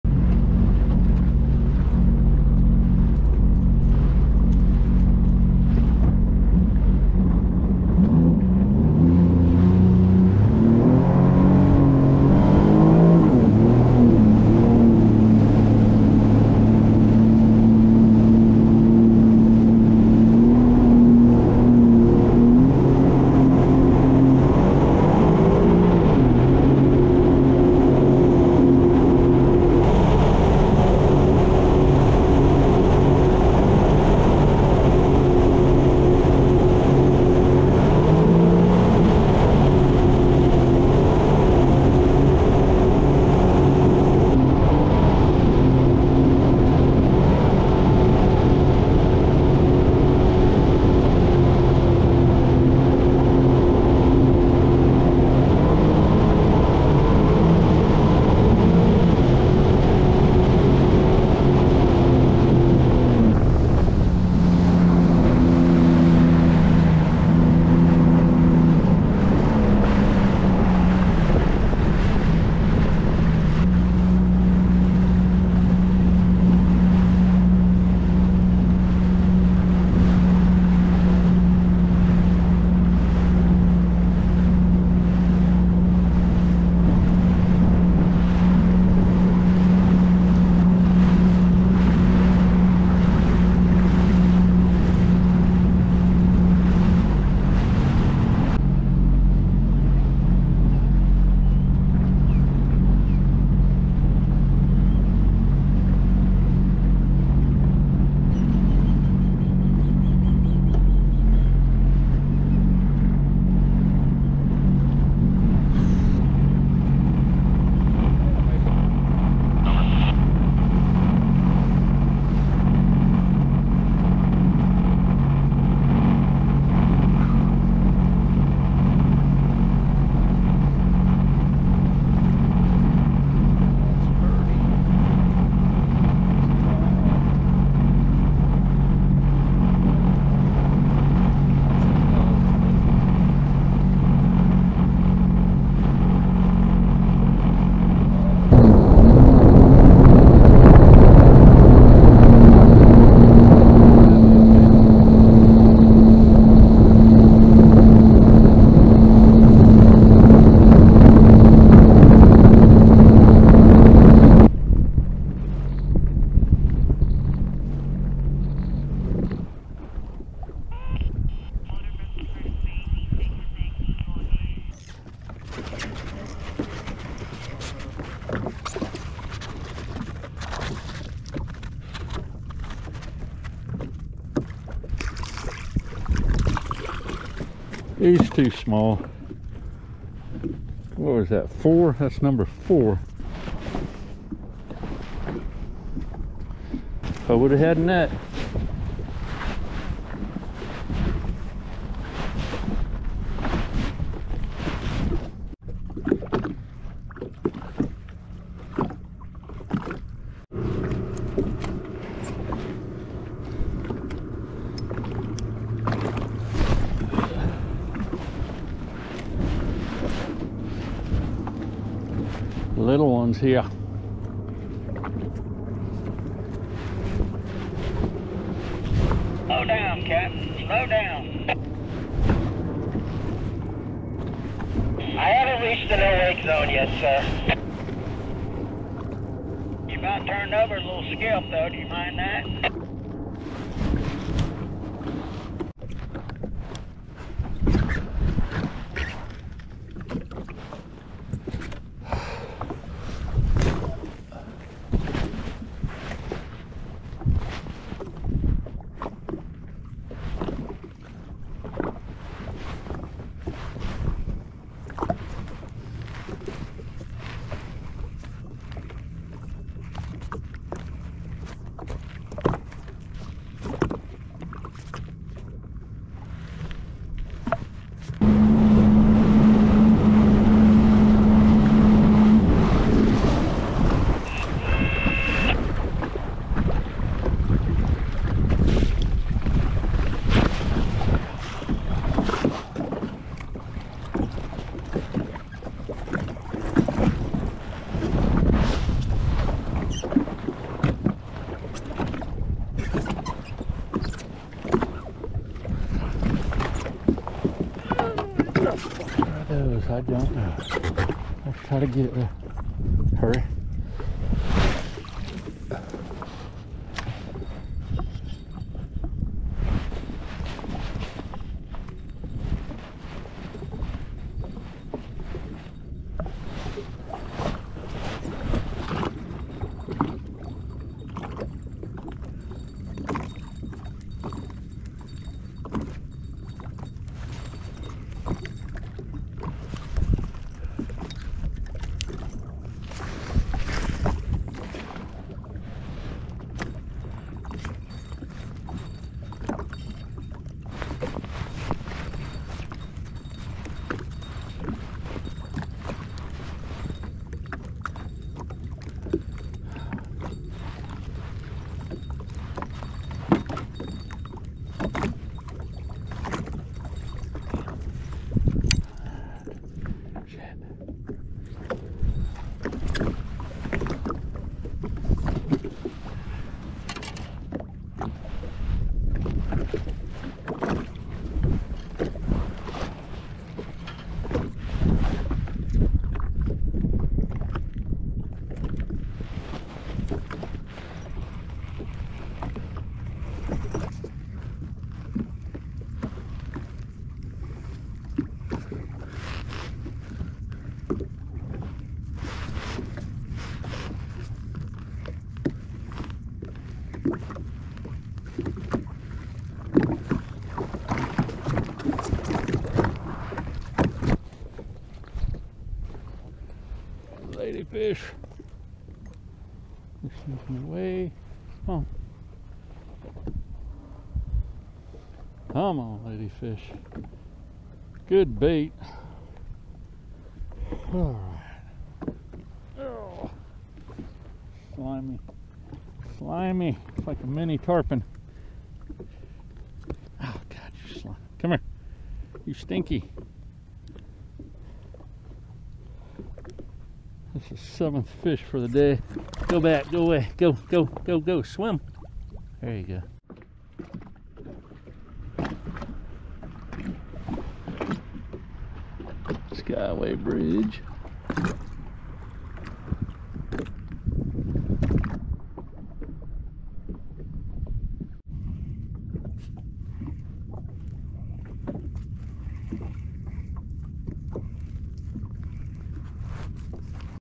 Jet Ski Fishing for Trout
Fishing Terra Ceia Bay and the mouth of Manatee River for Sea Trout on the Jet Ski...